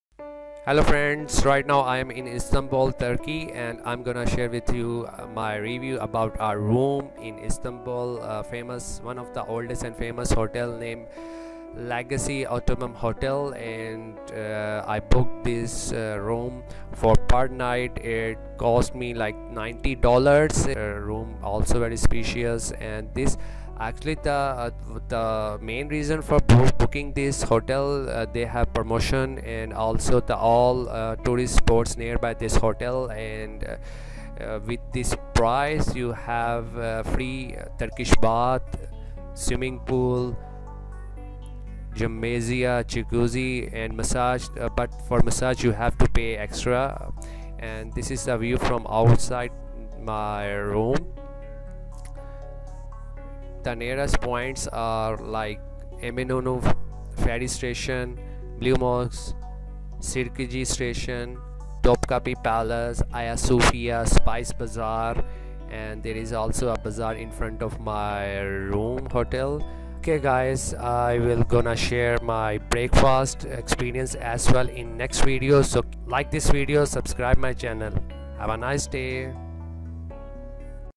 Legacy Ottoman Hotel Room Review l 5 Star Hotel 90$ per Night Only
I was stayed in Legacy Ottoman 5 Star Hotel just double bed room 90$ per Night with Breakfast and Free Turkish Bath,Swimming pool, Gym, also can take one guest with me. Amazing!! You can book hotel by using my following  Booking.com link .
free#LegacyOttomanHotel